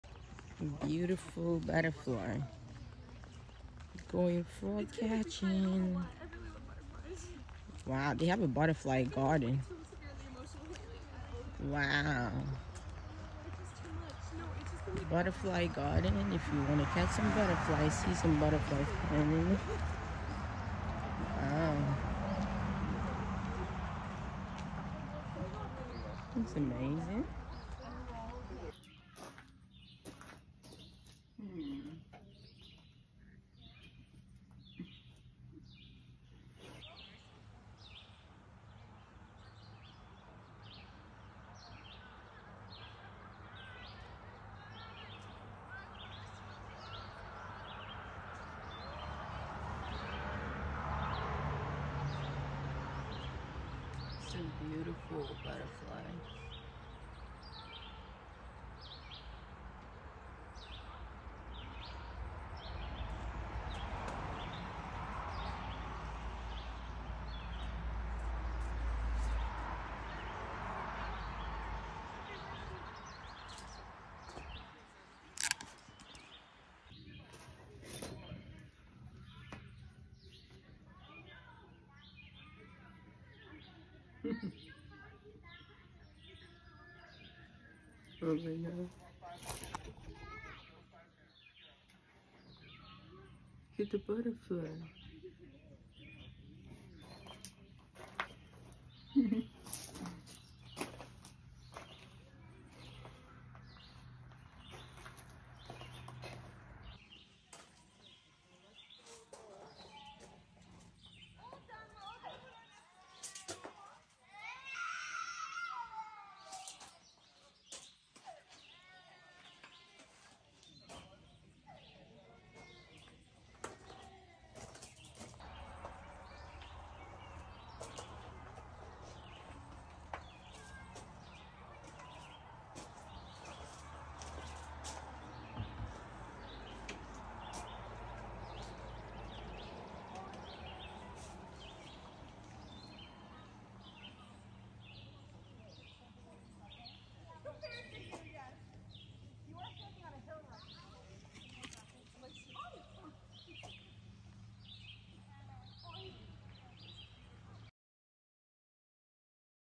We Visited The Butterfly 🦋 Garden￼-Vacation Adventure || Family Summer Vacation-Beautiful Butterfly🦋
Butterfly 🦋 Garden was our Adventure Time to day on vacation